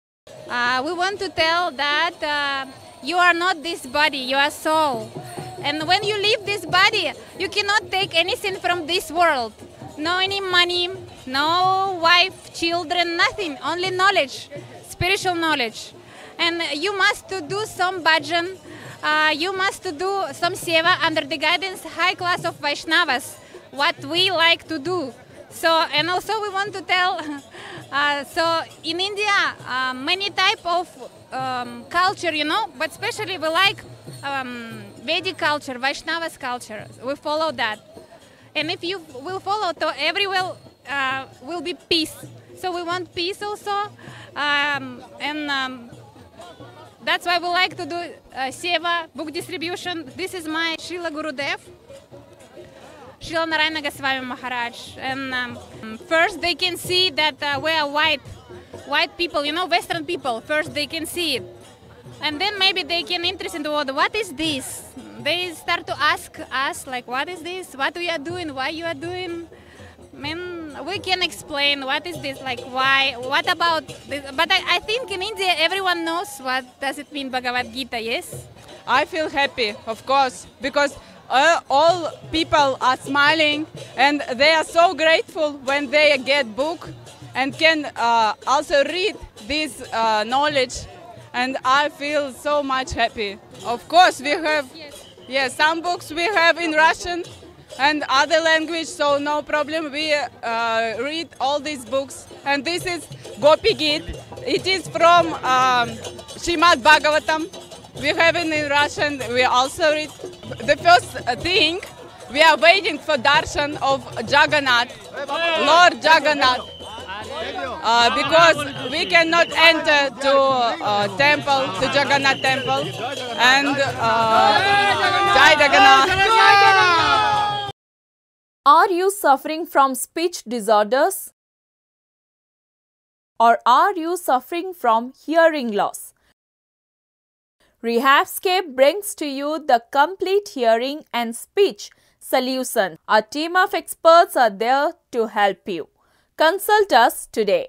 ବହି ବାଣ୍ଟିଲେ ବିଦେଶିନୀ
ଆଜି ପବିତ୍ର ଘୋଷଯାତ୍ରା । ବଡଦାଣ୍ଡରେ ଲୋକାରଣ୍ୟ ପରିବେଶ । ଏହାରି ମଧ୍ୟରେ ଲୋକଙ୍କୁ ଶ୍ରୀ ଗୋପିଗୀତ ବହି ବାଣ୍ଟିରେ କିଛି ବିଦେଶିନୀ।